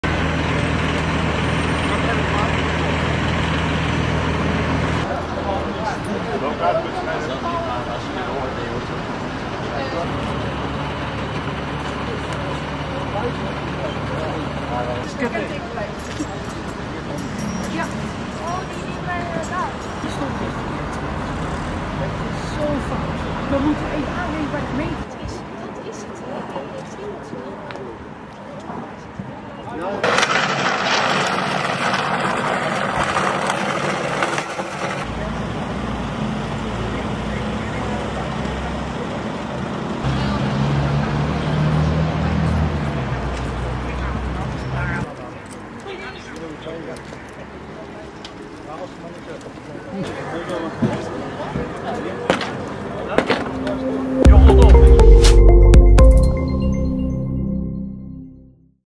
Veel schade na aanrijding met tram Statenlaan Den Haag
Den Haag - Donderdagavond 16 november heeft er op de Statenlaan kruising Frederik Hendriklaan een aanrijding plaatsgevonden tussen een auto en tramlijn 17 van HTM.

Meer info en foto’s in ons item op onze website of via onze District8-app